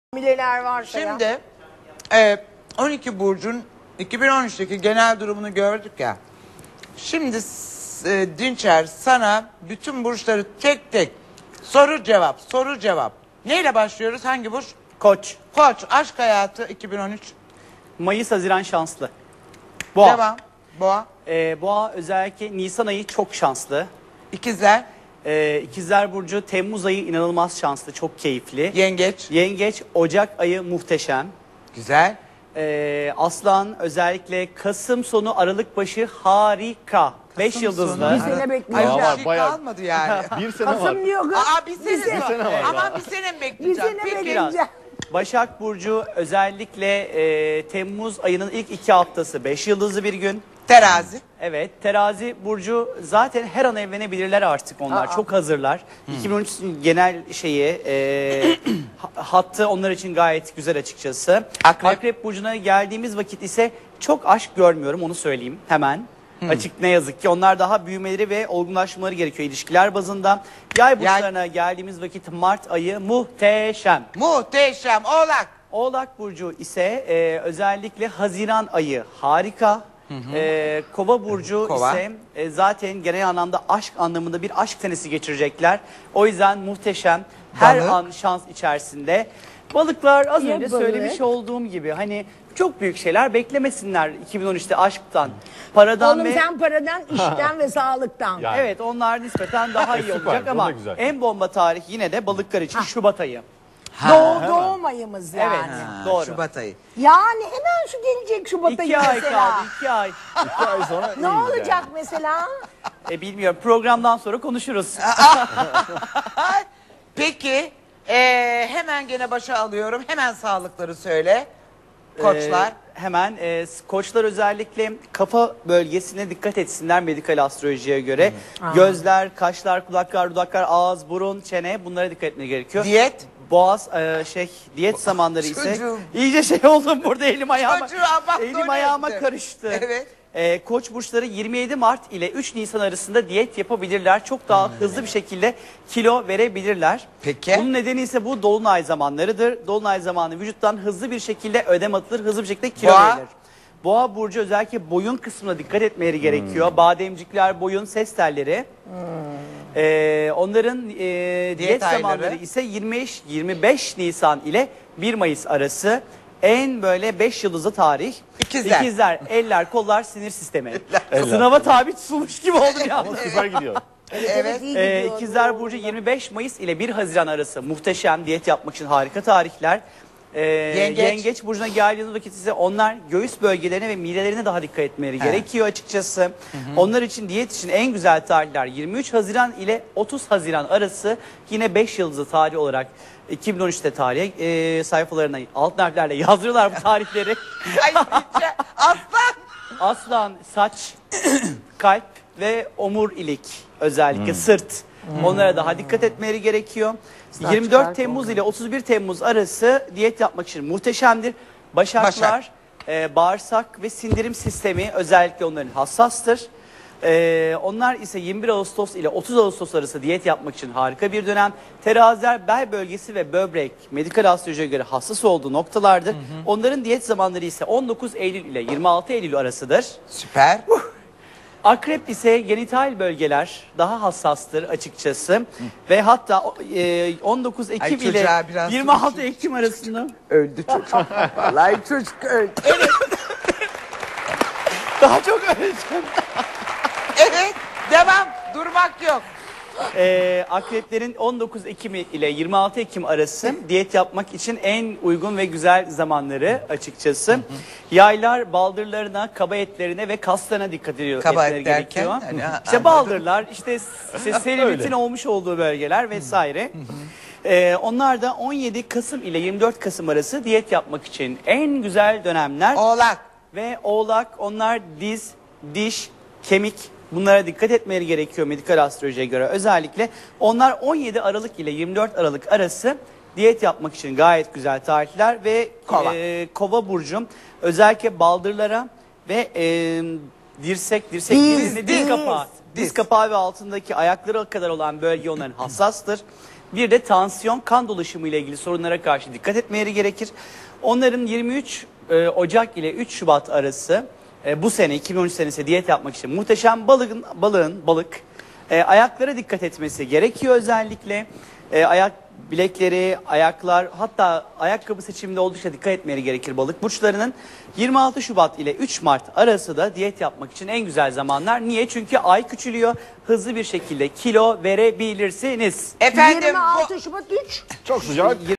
2013 yılında hangi burcu neler bekliyor?
Astrolog Dinçer Güner, 2013 yılında hangi burçları nelerin beklediğini açıkladı.